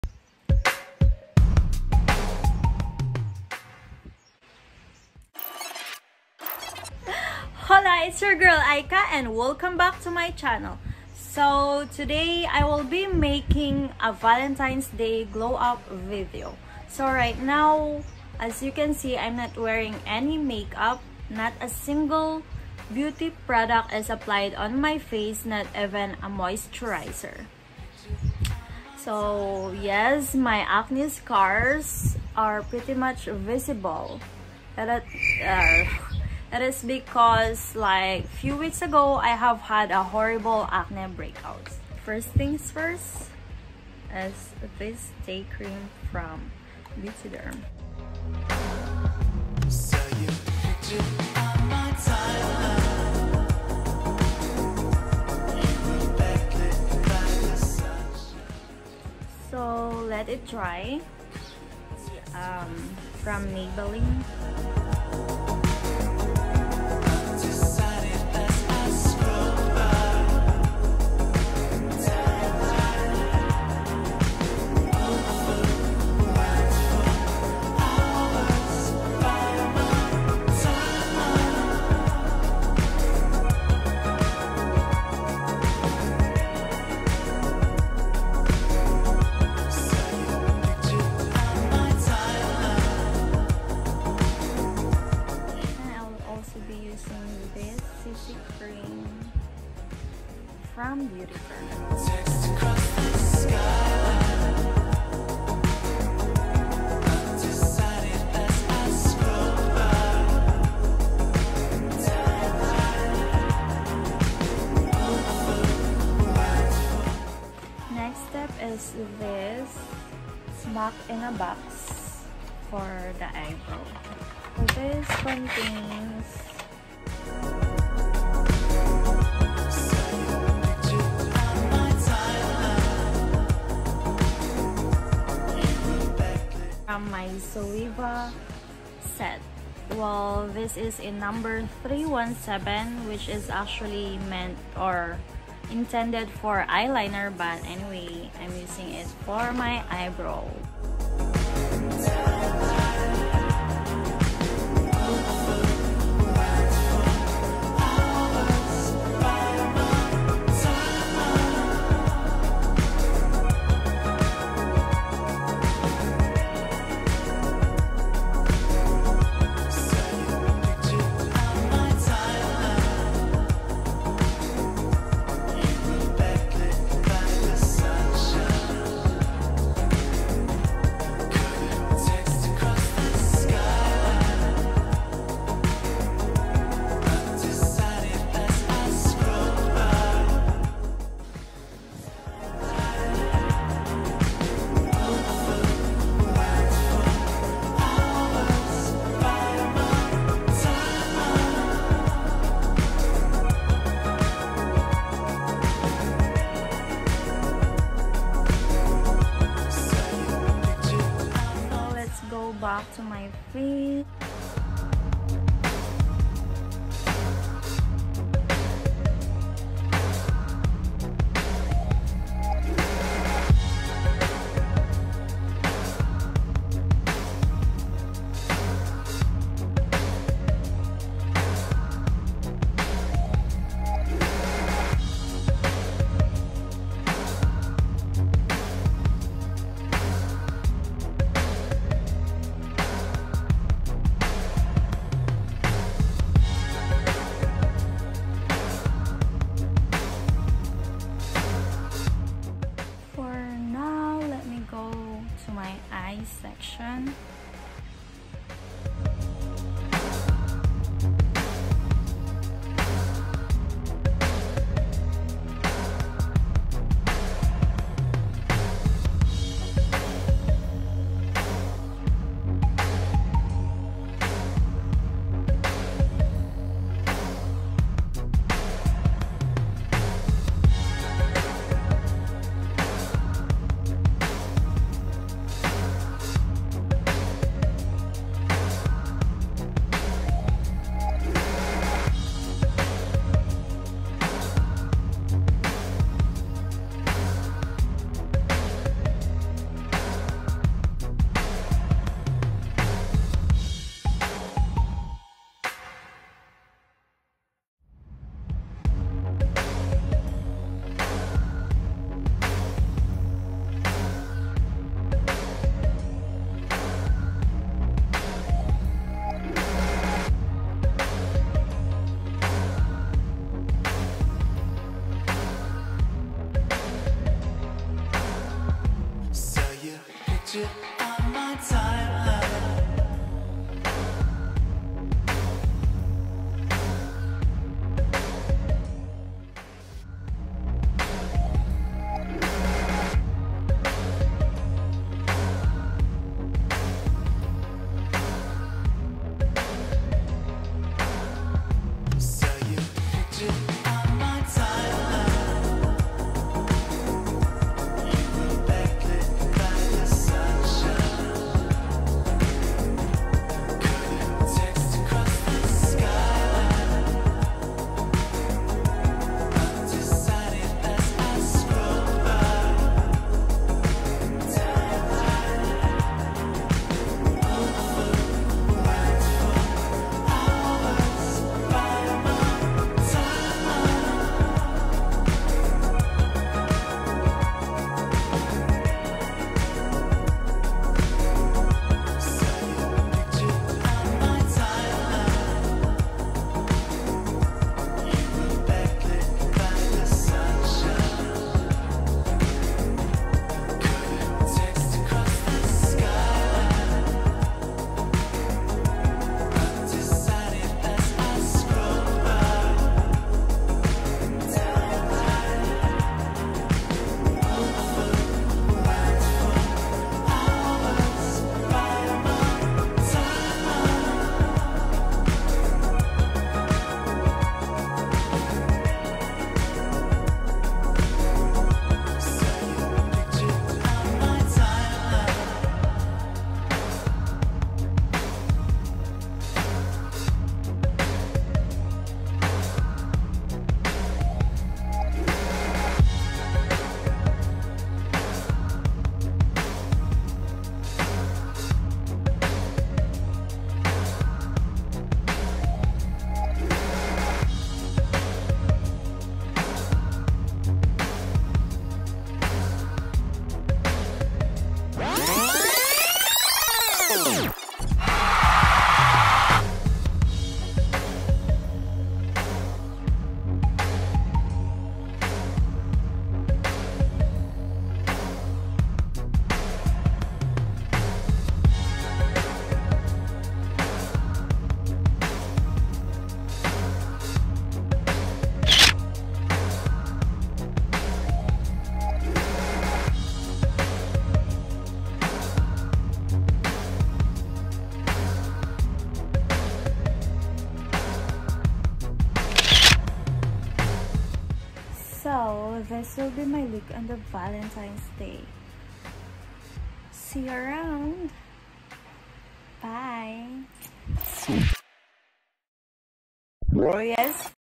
Valentine’s Day Glow Up | Ica
Products Used:
Day Cream: Beautèderm
Matte Poreless Foundation: Maybelline
CC Cream: CC Creme by Beautederm
Brow Powder: Mac
Brush: Zoeva #317
Brow Gel: Watsons
Kabuki Brush: Beautèderm
Sunscreen: Belo Tinted Sunprotect
Eye Palette/Blush: Smoky & Glittery Bag (Unknown; Gift from US)
Eyeliner Pen: Clinique
Eyeliner Pencil: Estee Lauder
Maskara: Clinique
Eyelash Curler: Althea Korea
Finishing Powder (Panda): Tony Moly
Liptint: Deep & Pure from Pony Effect
Countour/Highlighter: Maybelline V Dou

P.S. Sorry for some mispronounced words, I wasn’t conscious about it when I was filming. Thank you for your understanding 🤗